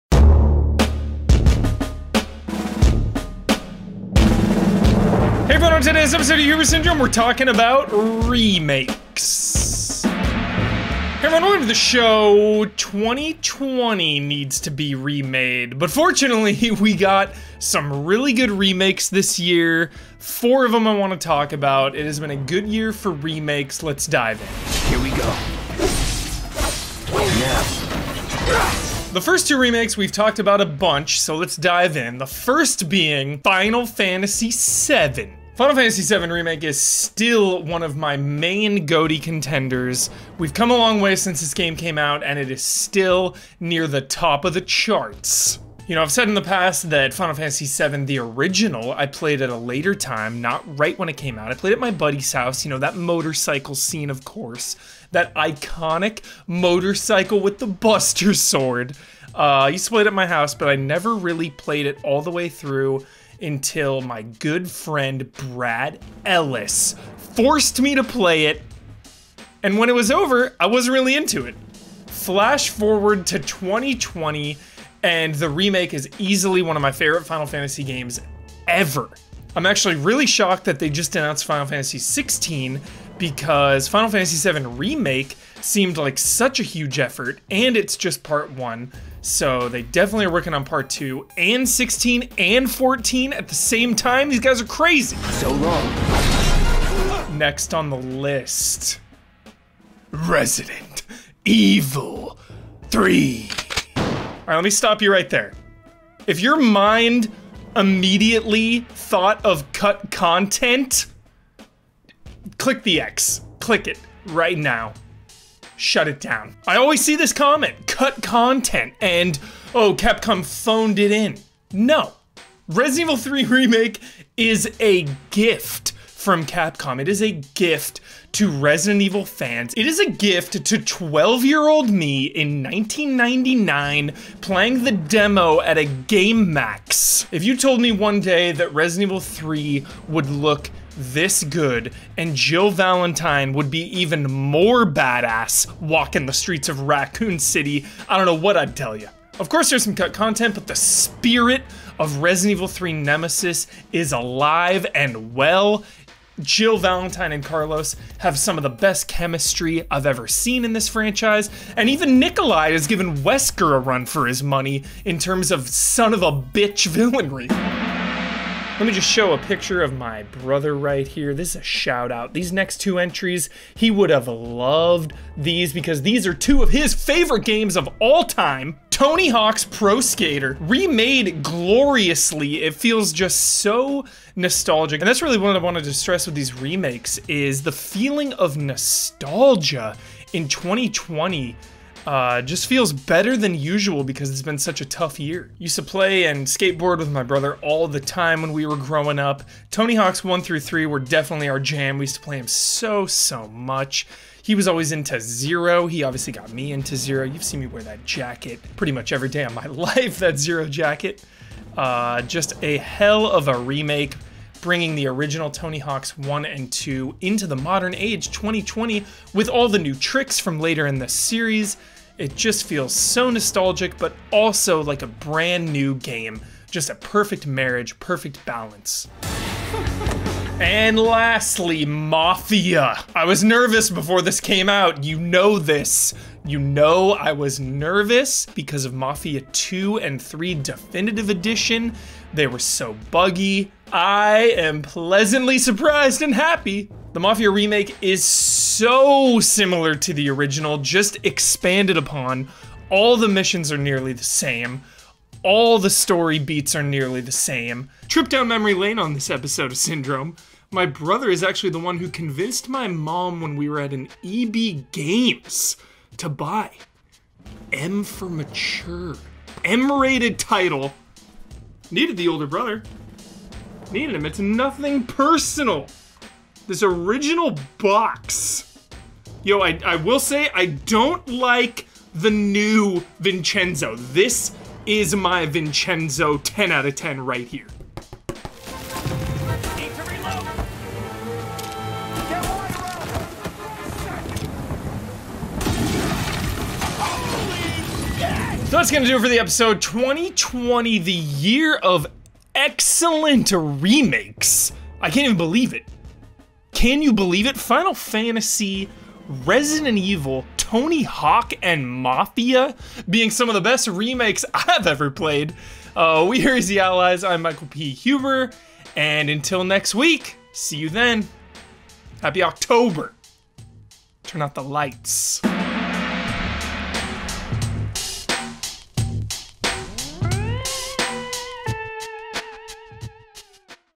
Best Remakes of 2020
Huber takes a nostalgic look back at 2020’s best remakes.